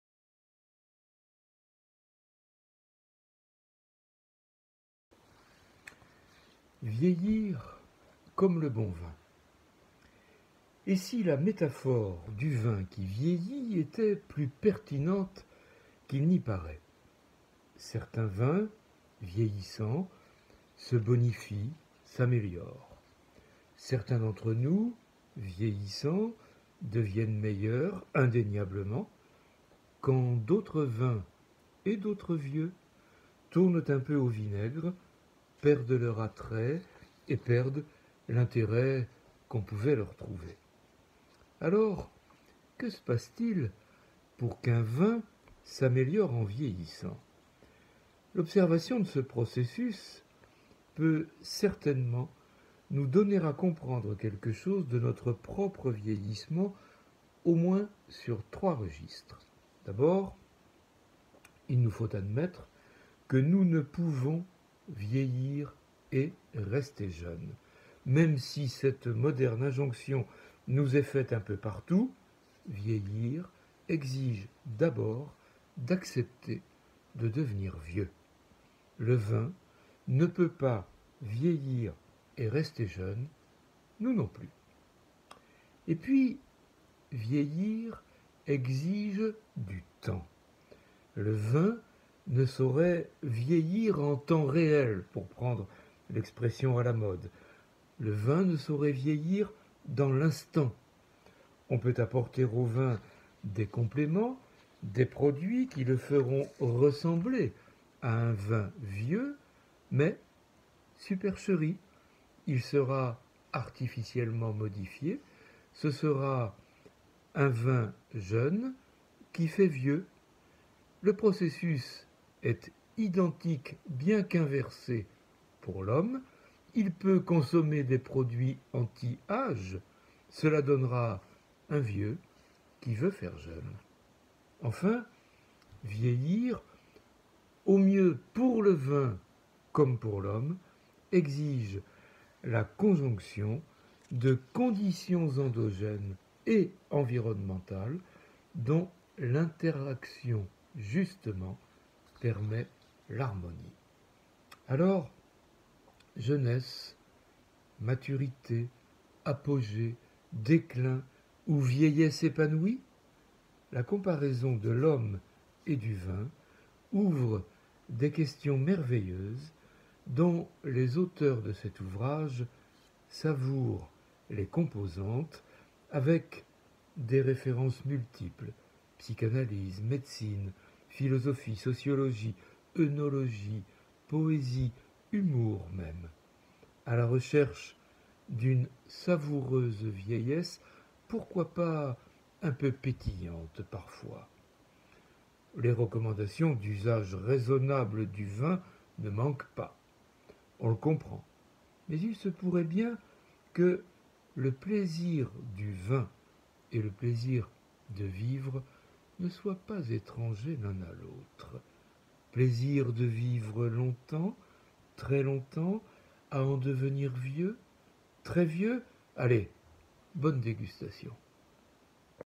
Michel BILLE - Vieillir comme le bon vin
Et si la métaphore du vin qui vieillit en disait plus long qu’il n’y paraît ? Pour le vin, vieillir exige des interactions avec des conditions environnementales… Ne serait-ce pas la même chose pour l’homme ?

Le vin vieillit-il comme nous ? Vieillissons-nous comme le vin ? Est-ce que nous nous bonifions ou au contraire nous gâtons-nous ? Est-on bien sûr que le vin ou les hommes s’améliorent toujours avec l’âge ?

Médecin, œnologue, psychanalyste, sociologue, philosophe en disent quelque chose et s’intéressent évidemment à ce lien possible entre la vieillesse des humains (hommes et femmes) et celle du vin, comme si cette dernière, peut-être, pouvait nous guider, nous donner des repères, des idées, nous aider à penser notre vieillesse, notre avancée en âge, et pourquoi pas à la penser comme une bonification continue.

« Vieillir comme le bon vin » pourrait devenir alors une sorte de défi à relever, pour celles et ceux qui semblent parfois douter de ce qui pourrait leur arriver de bon dans l’avancée en âge. Il se pourrait même que cette réflexion nous conduise à ouvrir, avec le sourire mais très sérieusement pourtant, des questions sur le sens de ce que nous vivons en vieillissant et faisons vivre parfois à celles et ceux qui vieillissent, des questions éthiques finalement !

Vivre, acquérir de la maturité, devenir meilleur, « prendre de la bouteille », tout simplement…

Alors, vieillir comme le bon vin et avec le bon vin, avec du bon vin pour sentir, goûter, partager, savourer, déguster, apprécier, s’émerveiller, et vivre, vivre, vivre encore…